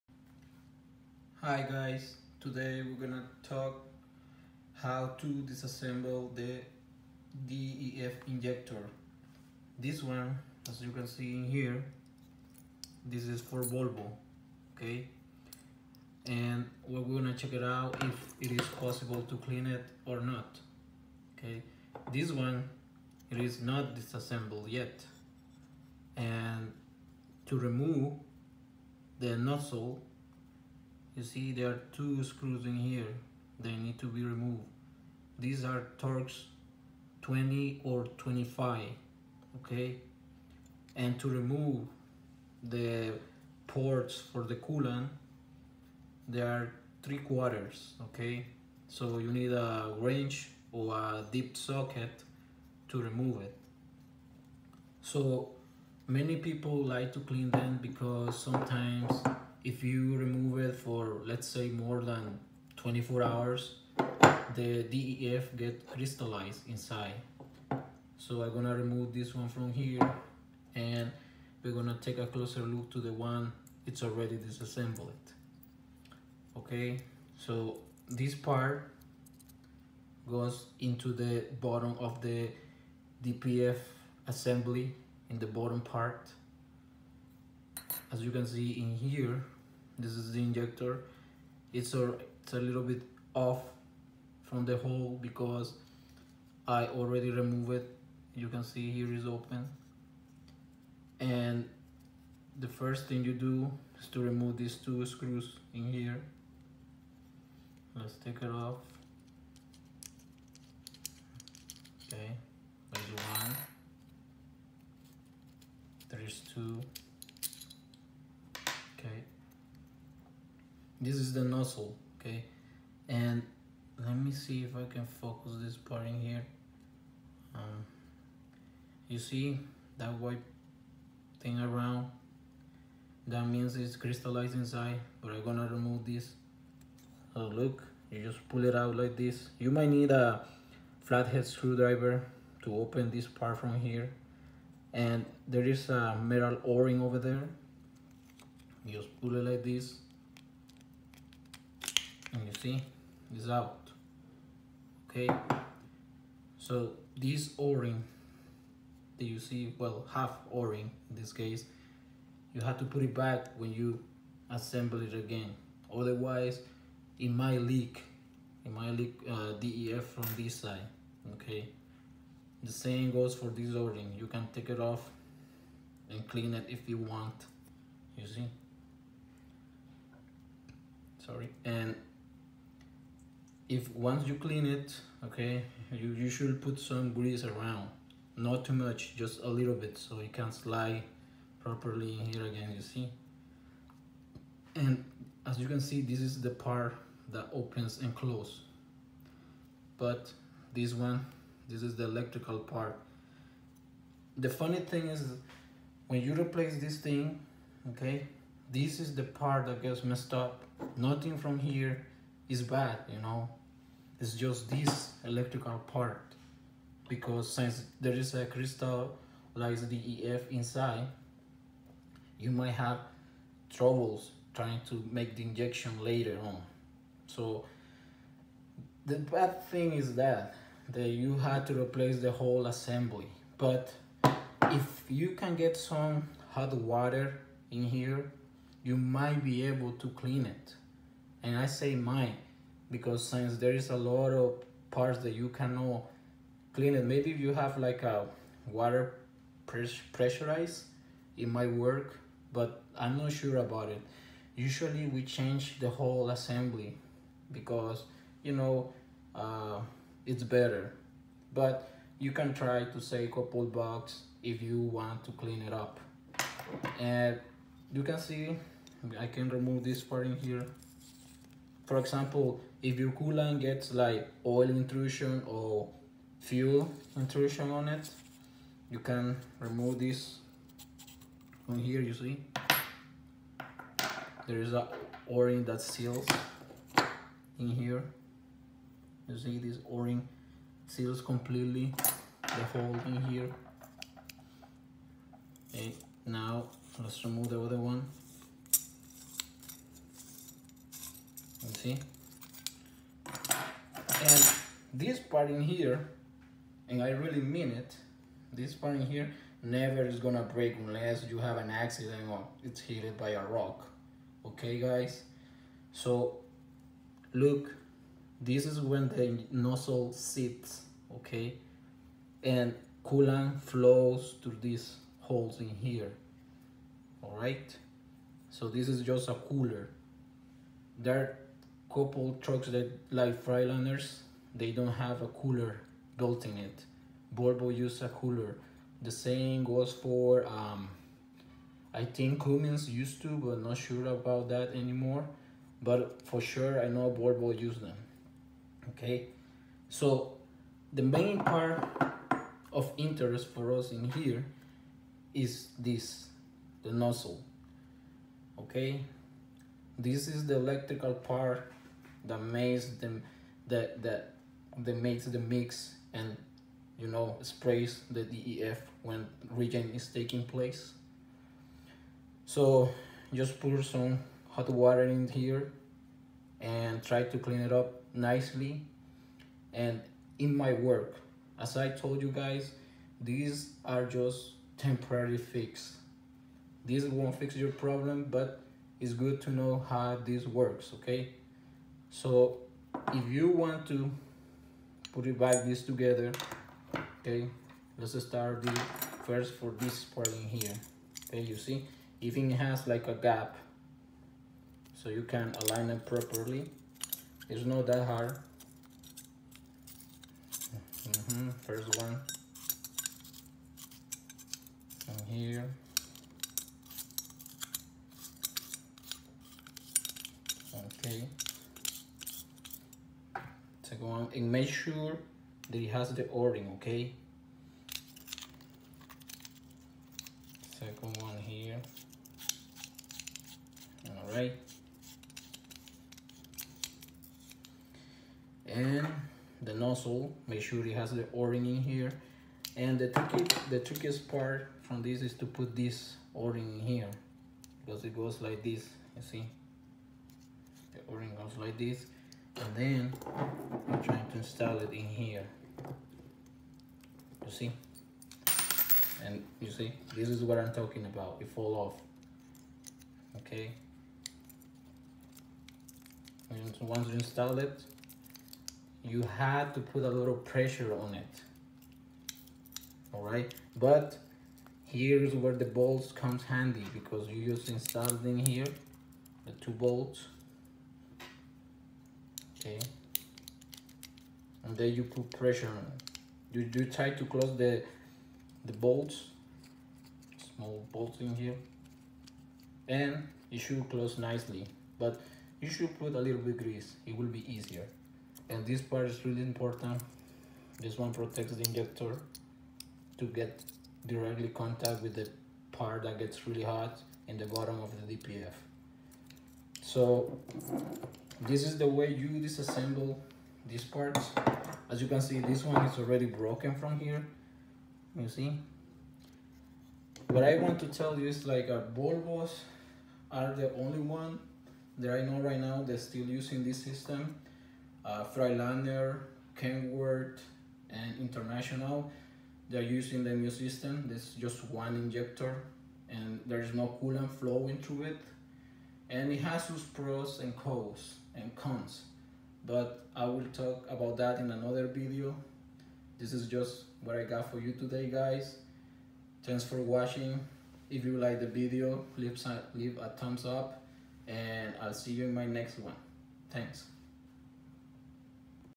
Assembly/Disassembly DEF injector (Volvo applications).
Welcome again to Let’s talk DIESEL. We are about to go really in deep and get a closer look to DEF (Diesel Exhaust Fluid) injector, is it possible to repair it? Or perhaps it is better to change the whole thing?... Let’s find out people. Have a question? Share it in the comments below.